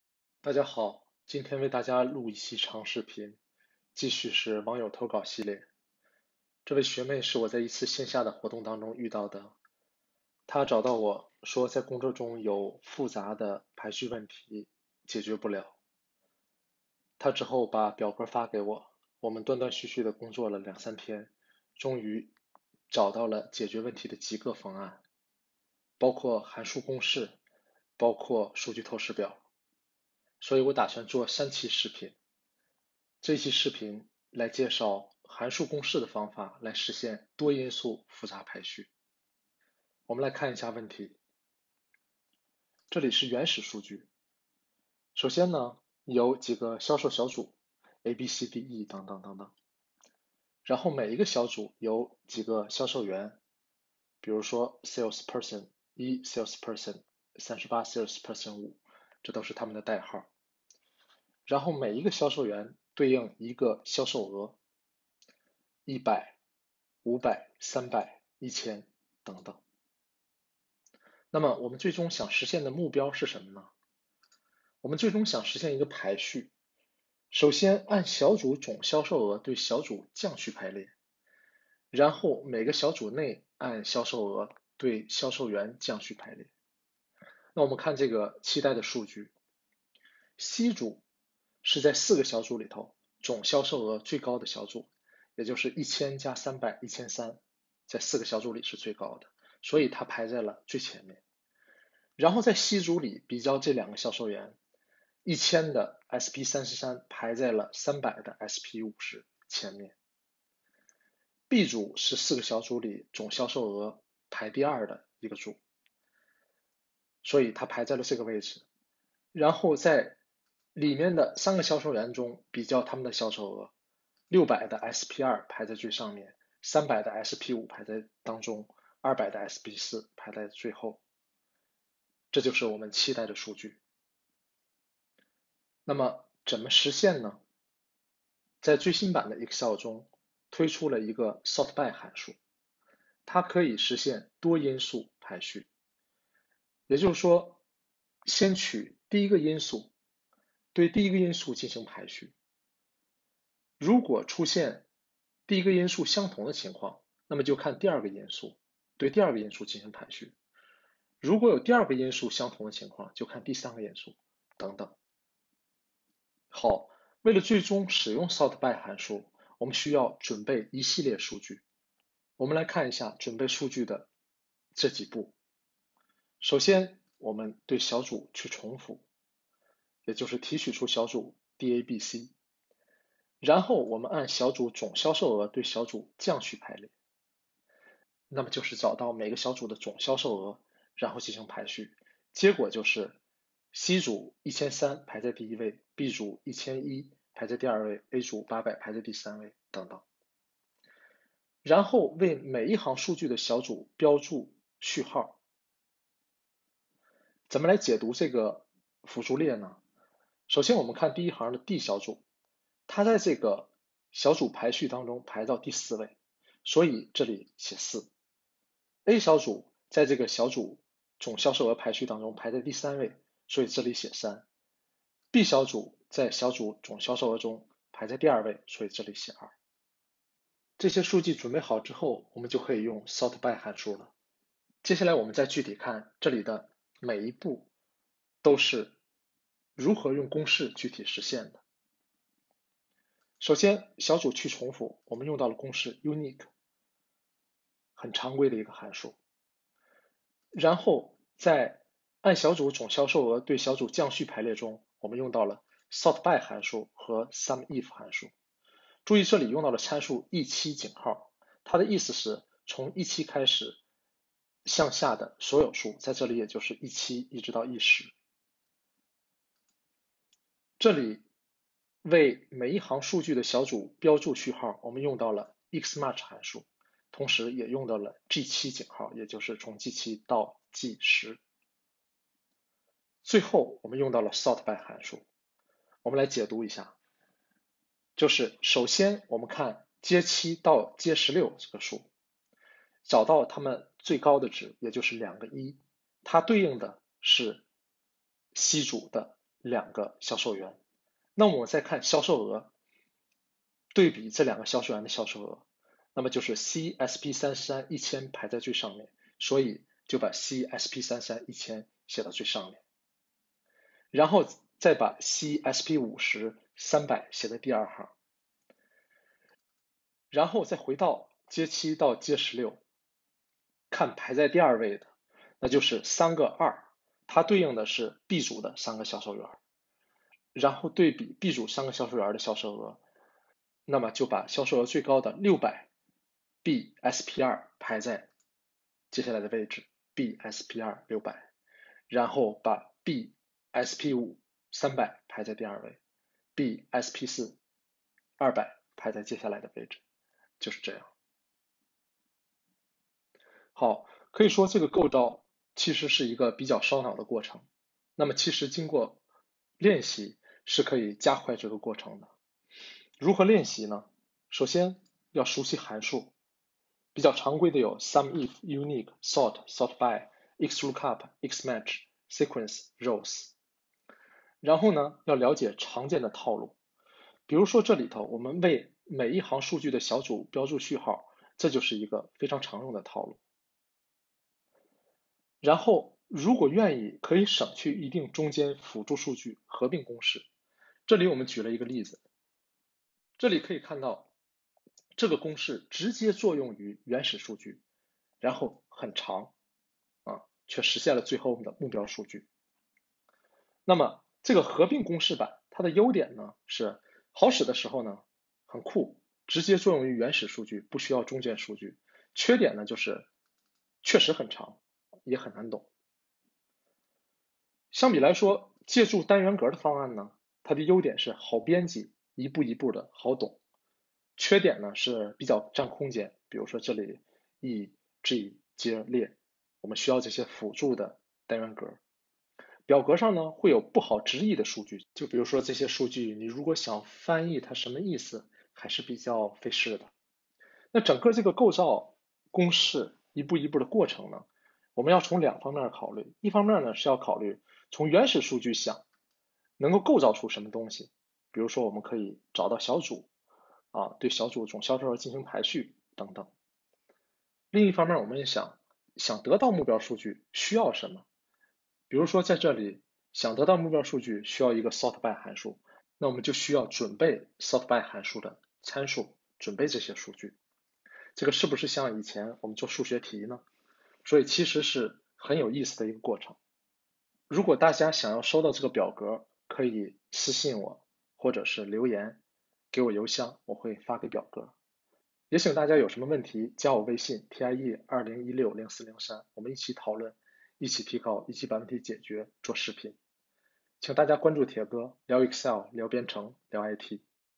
【网友投稿】【长公式】手把手教你构造长公式，实现多因素复杂排序
长公式